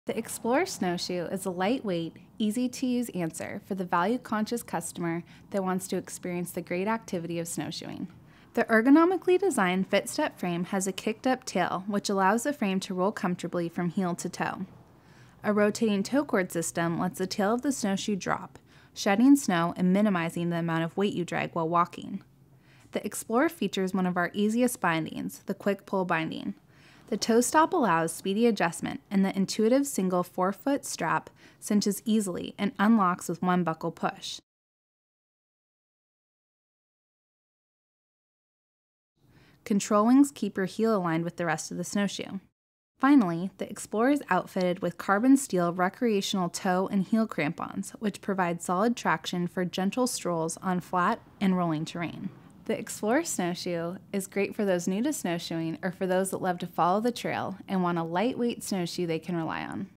Xplore
The new QuickPull Binding on the Xplore make this snowshoe easy to get into and comfortable for a day out in the snow.